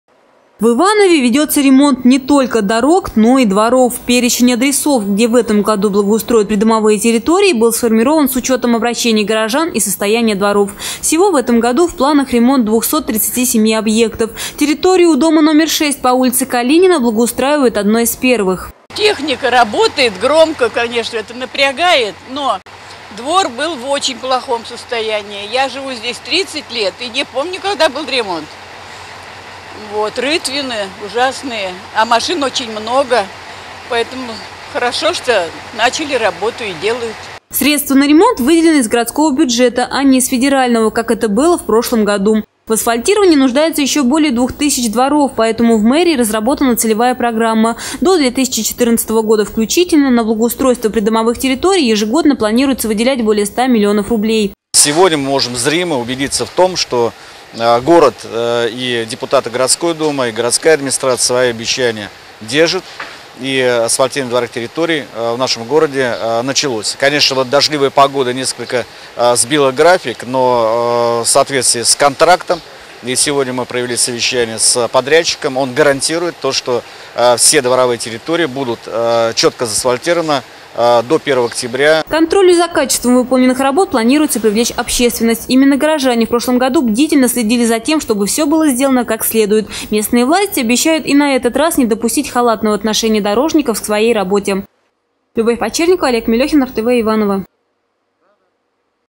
Ремонт дворов
В Иванове ведется ремонт не только дорог, но и дворов. Перечень адресов, где в этом году благоустроят придомовые территории,  был сформирован с учетом обращений горожан и состояния дворов.